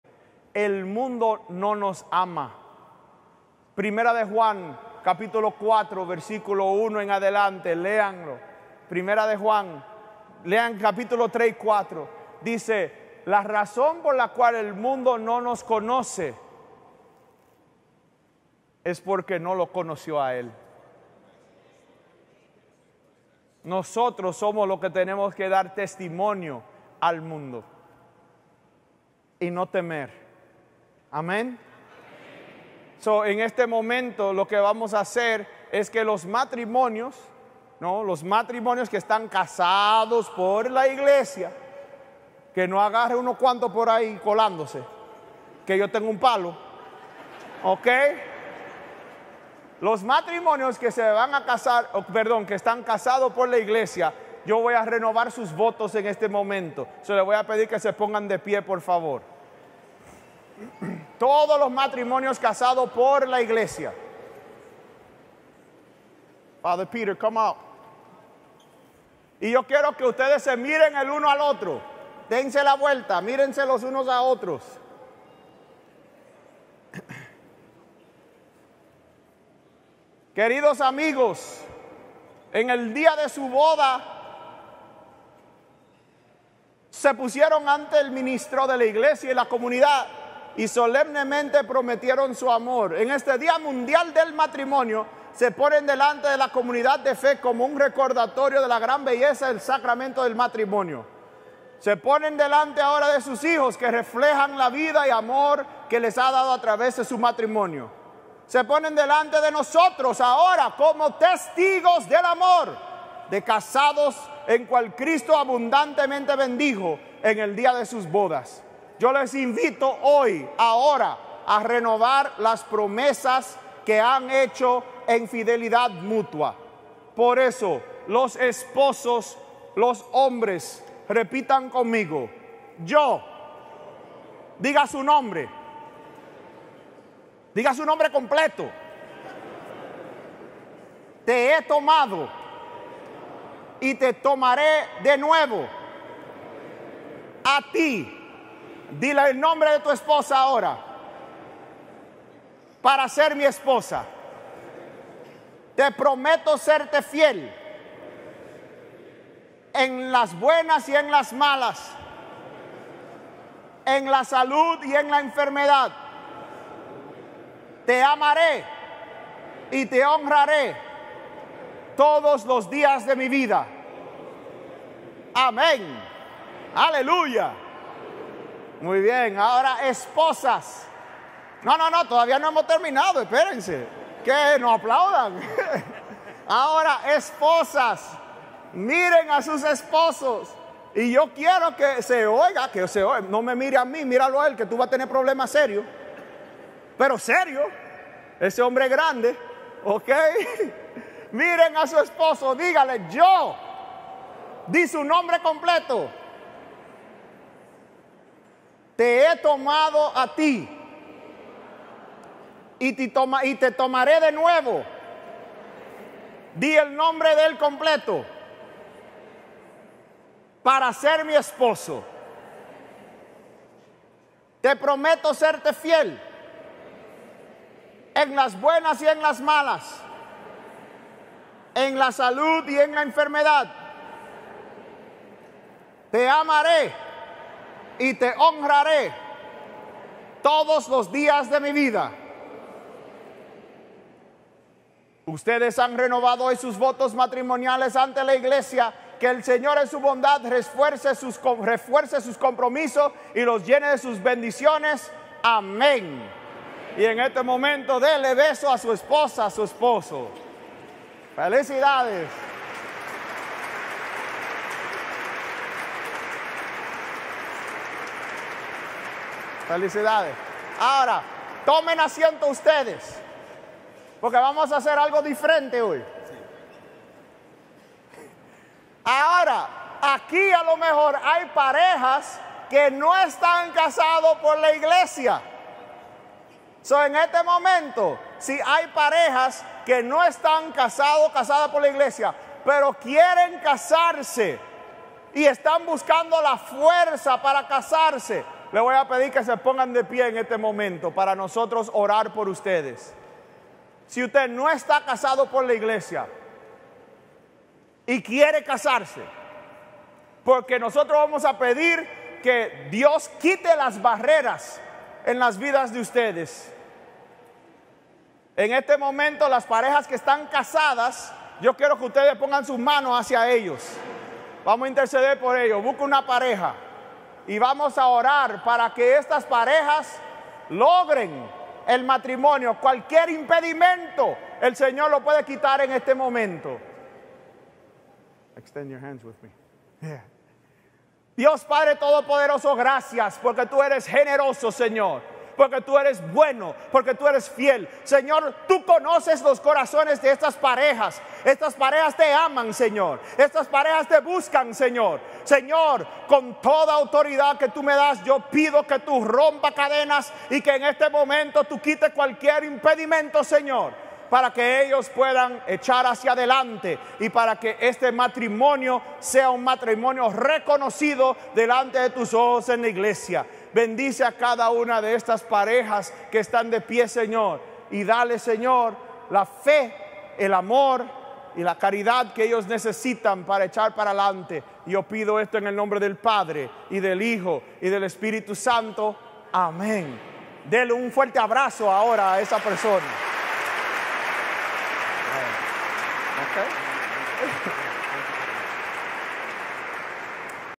RENOVACION DE VOTOS MISA DEL DIA MUNDIAL DEL MATRIMONIO 2023
LA RENOVACION DE LOS VOTOS MATRIMONIALES Y ADEMAS LA ORACION POR LAS PAREJAS QUE ESPERAN POR EL SACRAMENTO DEL MATRIMONIO EN LA MISA DEL DIA MUNDIAL DEL MATRIMONIO CELEBRADA EL 12 DE FEBRERO DEL 2023 POR MONSEÑOR JOSEPH ESPAILLAT EL LA CATEDRAL DE SAN PATRICIO EN NEW YORK